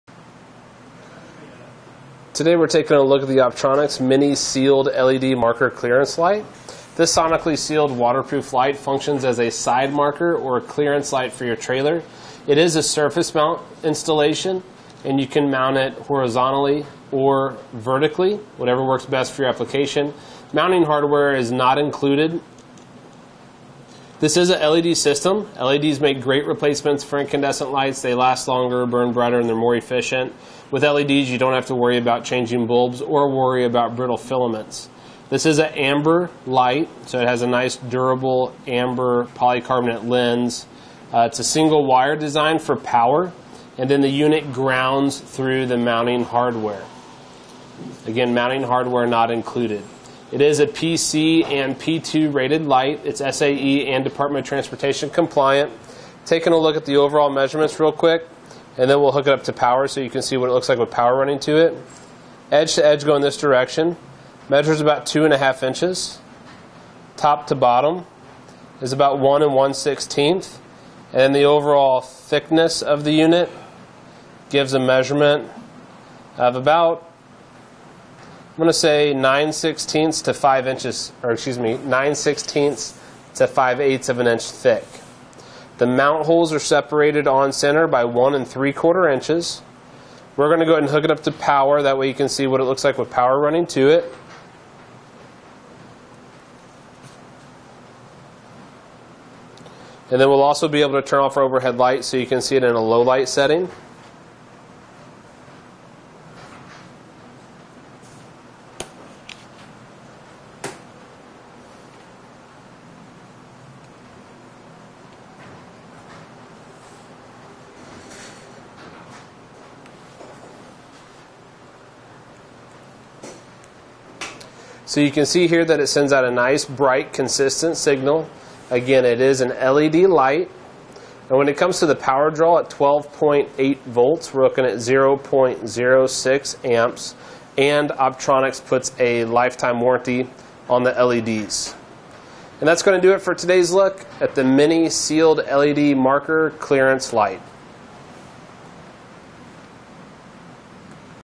etrailer | Comprehensive Review: Optronics Mini LED Clearance or Side Marker LIght
Speaker 1:          Today we're taking a look at the Optronics Mini Sealed LED Marker Clearance Light. This sonically sealed waterproof light functions as a side marker, or clearance light for your trailer. It is a surface mount installation. You can mount it horizontally, or vertically, whatever works best for your application. Mounting hardware is not included. This is a LED system.

LEDs make great replacements for incandescent lights. They last longer, burn brighter and they're more efficient. With LEDs, you don't have to worry about changing bulbs or worry about brittle filaments. This is an amber light, so it has a nice, durable amber polycarbonate lens. It's a single wire design for power.

And in the unit grounds through the mounting hardware.  Again, mounting hardware not included.It is a PC and P2 rated light. It's SAE and Department of Transportation compliant. Taking a look at the overall measurements real quick, and then we'll hook it up to power so you can see what it looks like with power running to it. Edge to edge going this direction measures about two and a half inches.

Top to bottom is about one and one sixteenth, and the overall thickness of the unit gives a measurement of about, I'm going to say nine sixteenths, to five inches. Or excuse me, nine sixteenths to five eighths of an inch thick.The mount holes are separated on center by one an three quarter inches. We're going to go ahead and hook it up to power. That way you can see what it looks like with power running to it. And then we'll also be able to turn off overhead light so you can see it in a low light setting.

So you can see here that it sends out a nice, bright, consistent signal. Again, it is an LED light. But when it comes to the power draw at 12.8 volts we're looking at 0.06 amps and Optronics puts a lifetime warranty on the LEDs.And that's going to do it for today's look at the Mini Sealed LED Marker Clearance Light.